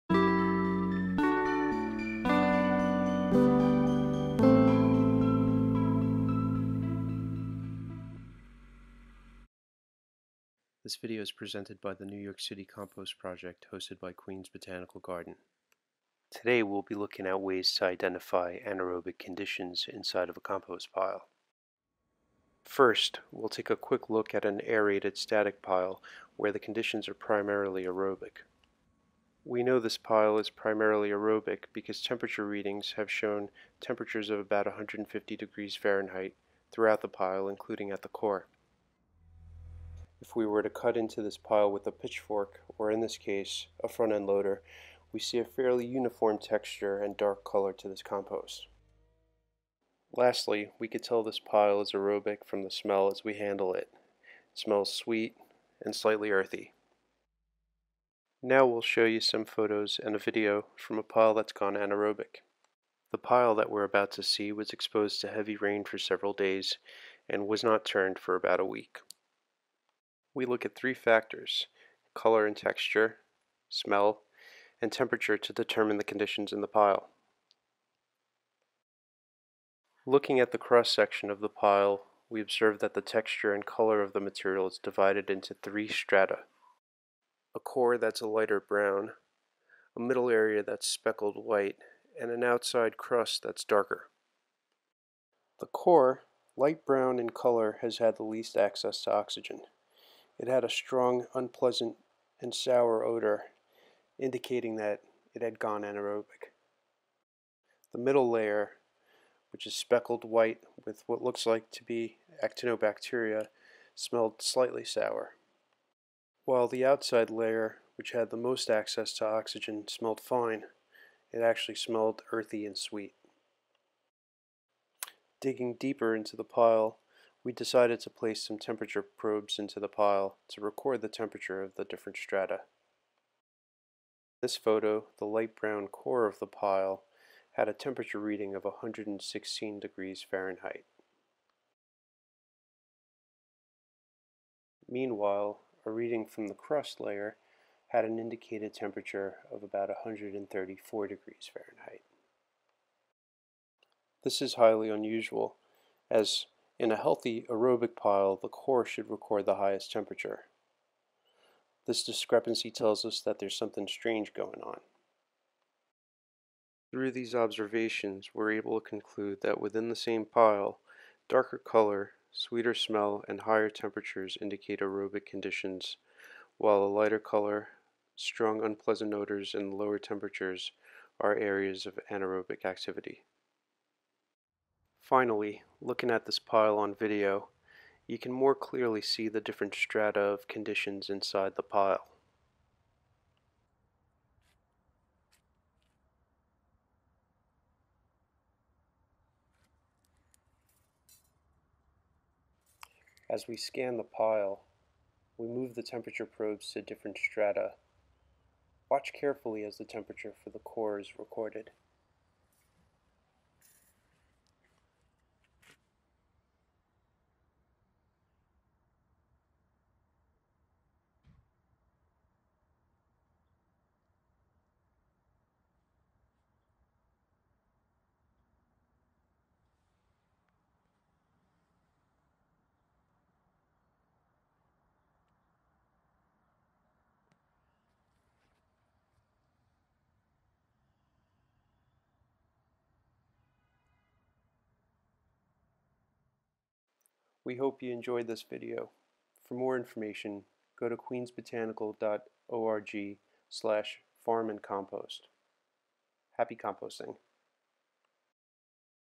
How to Identify Anaeroic Conditions in Your Compost Pile
This video looks at anaerobic conditions inside of a compost pile. We look at two piles, the first is aerobic and representative of our operations while the second pile due to specific circumstances has gone anaerobic. This material in the second pile is five weeks old and left uncovered during several days of rain.  It had not been turned in around 1 week.  The pile had several unusual characteristics: The temperature of the core of the pile was significantly lower than the outside of the pile, the material at the core smelled distinctively anaerobic while the outside was earthy, and the compost pile divided into three visible strata.